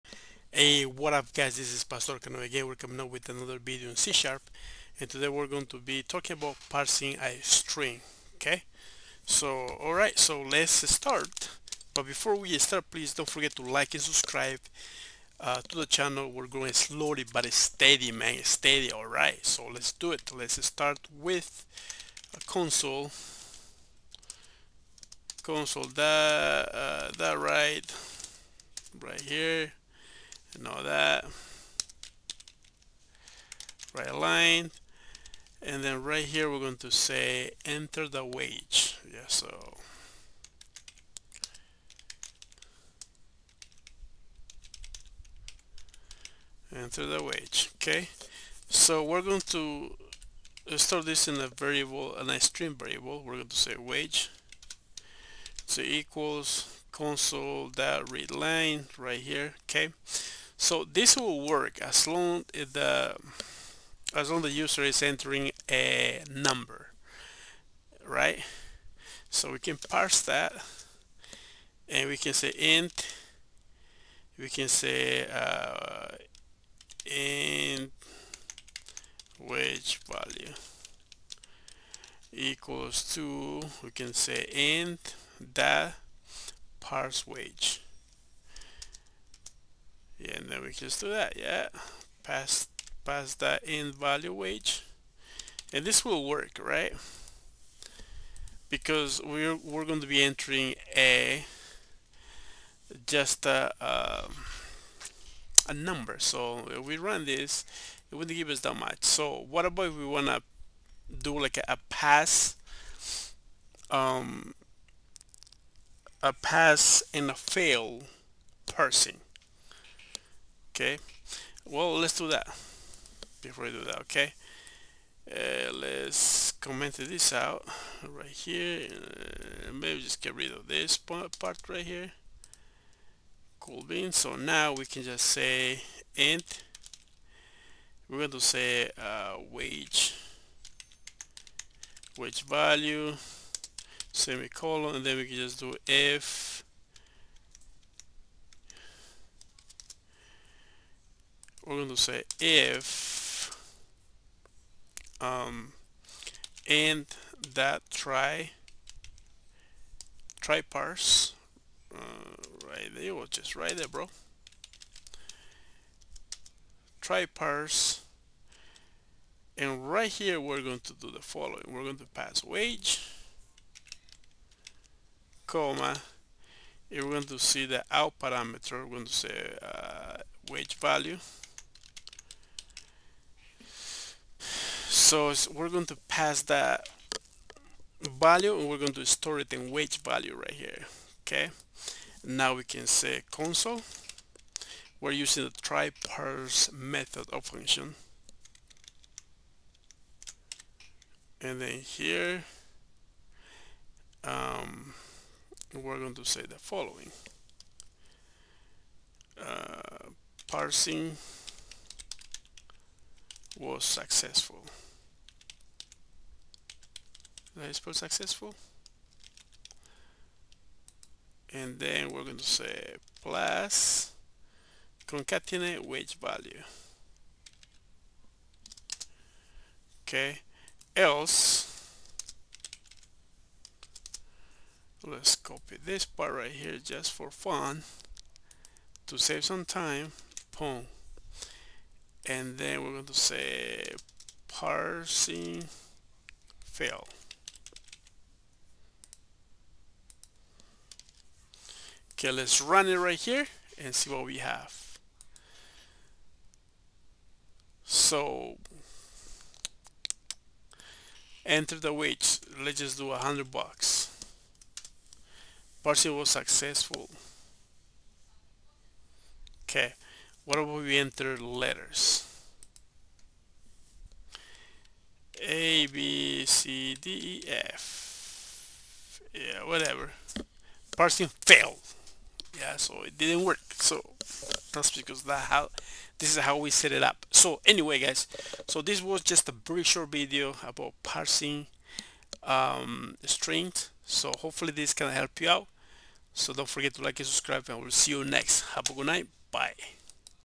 C# Programming - PARSING A STRING
This was a cool video to make.  Very simple example of how to parse an integer.  Hopefully this video helps you out.

Thanks.